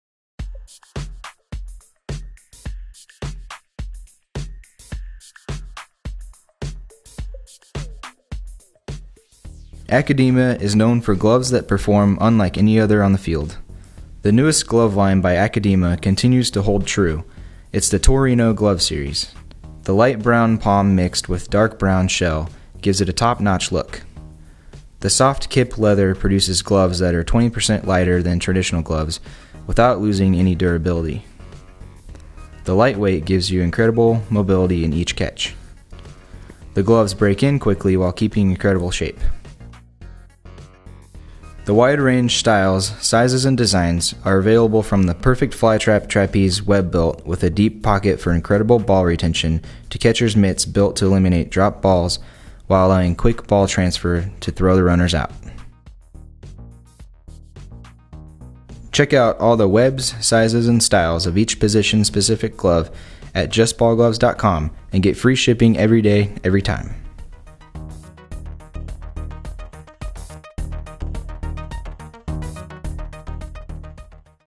Akadema Torino Glove Series - JustBallGloves.com
The Akadema Torino Series is available in multiple sizes, webs, and positions!